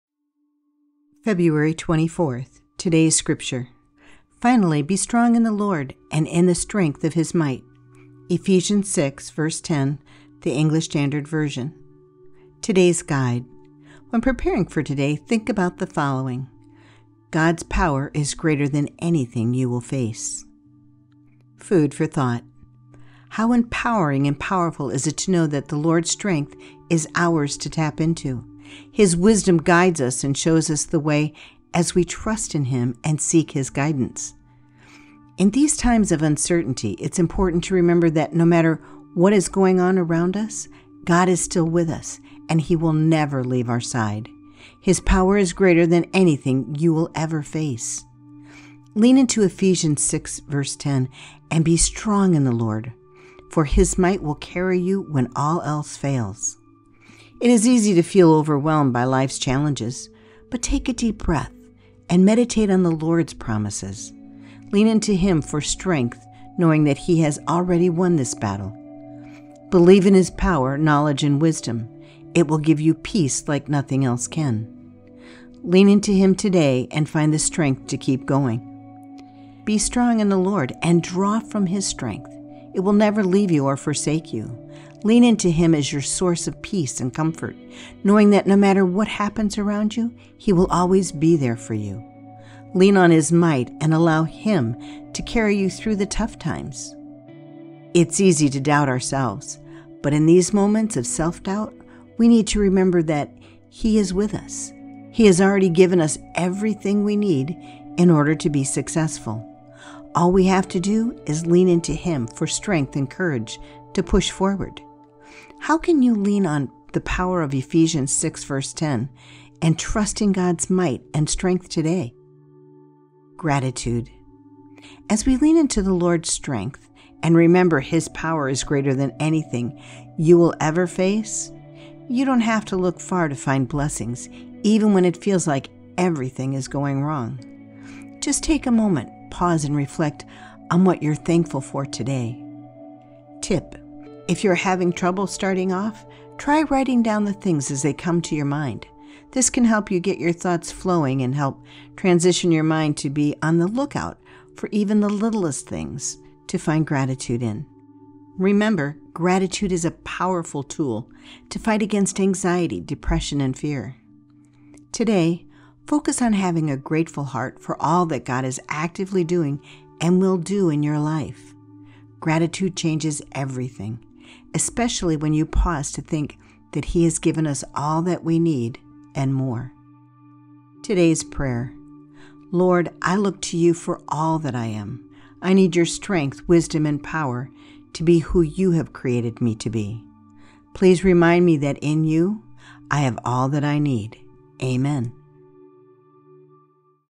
Today's Guide | February 24
Today’s Guide: God’s Power is Greater Than Anything You Will Face.

Today’s Scripture: Finally, be strong in the Lord and in the strength of his might.
Ephesians 6:10 ESV